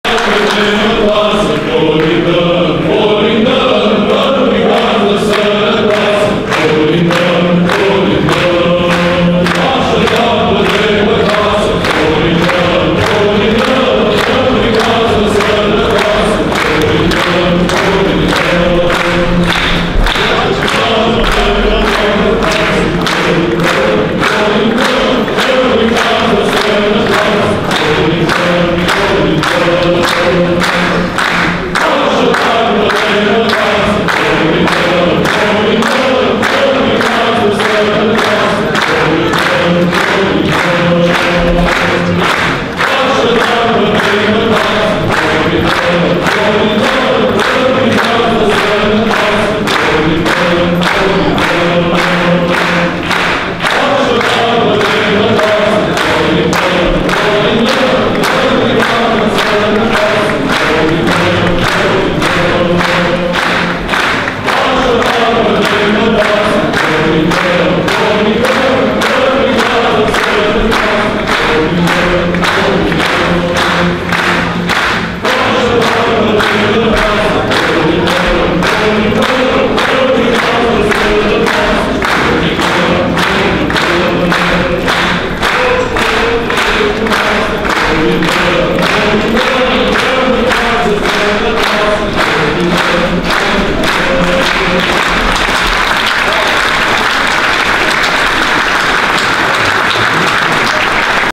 (33) Grupul psaltic TRONOS al Catedralei patriarhale - Cântare (2019.12.14, Sf. Constantin și Elena)
Sâmbătă, 14 decembrie 2019, începând cu ora 19.00, la Biserica Sfinții Împărați Constantin și Elena - Parcul Călărași (Piața Muncii), Grupul psaltic TRONOS al Catedralei patriarhale, dirijat de Părintele arhid. Mihail Bucă, ne-au invitat să trezim prin intermediul colindelor: dragostea, omenia, respectul, emoţia, empatia, lucruri care contează foarte mult.

Colindele au menirea de a face legătura între oameni și Dumnezeu care Se întrupează, între Împărăția Cerurilor şi pământ. Când se cântă colinde, cerul se deschide. Și tot astfel trebuie să se deschidă și inimile noastre către cei aflați în anumite nevoi sau suferințe.

La ediția din 2019 am fost alături de Alexandra. Despre cazul ei, am aflat mai multe în cadrul concertului caritabil.

Gazdă eveniment: Pr. Asist. Dr. Nicolae Giolu.